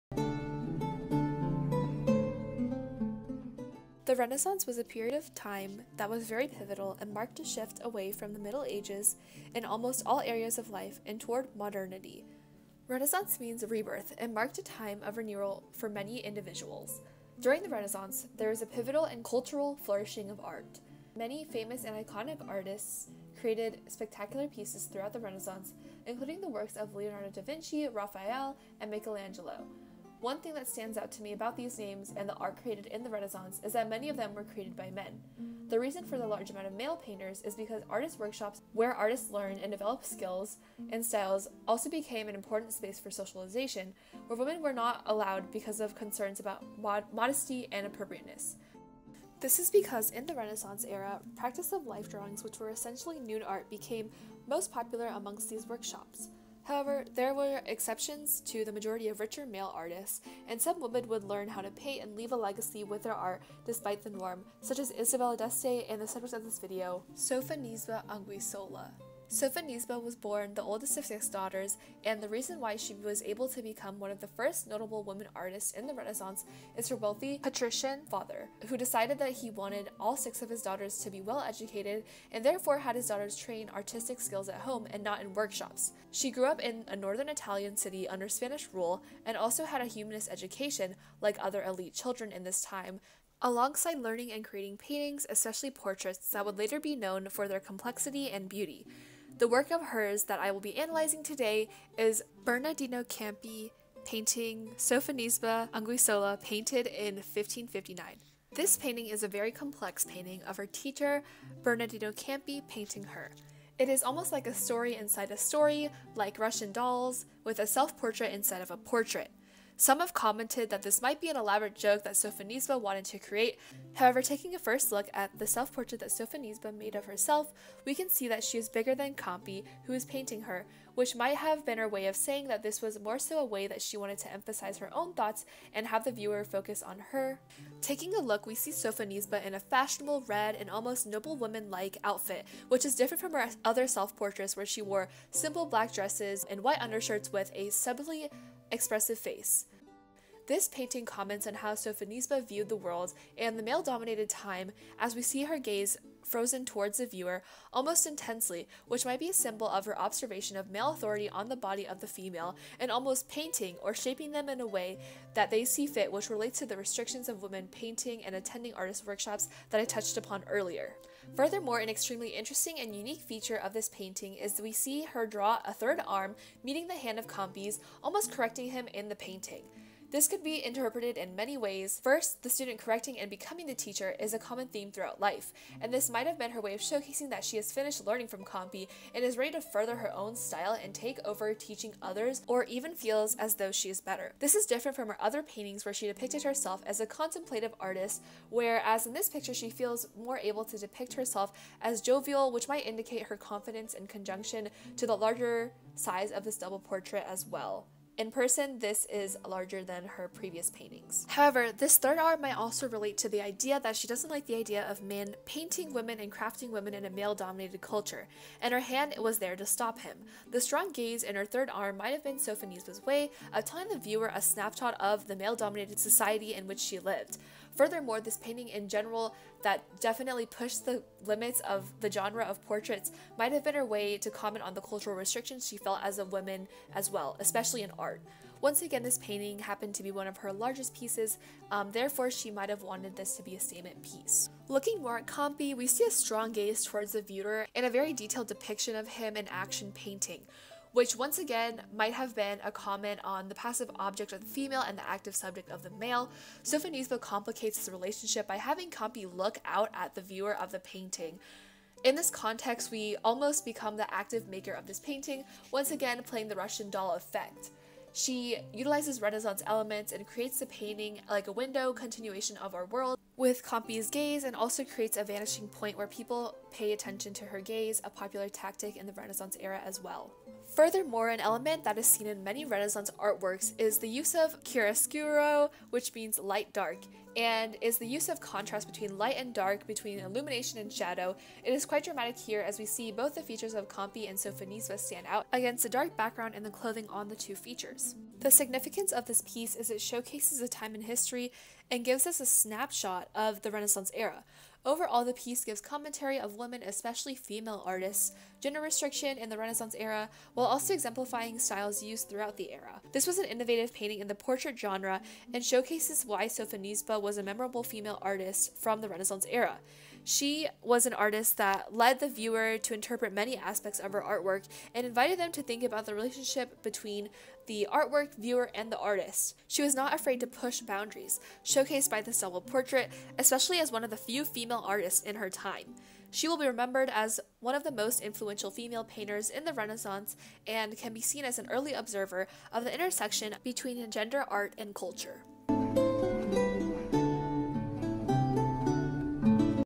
Italian 40 Midterm Project-Lucinda Cheng | Sofonisba Anguissola
My Italian 40 Project on one of Sofonisba Anguissola's self-portraits!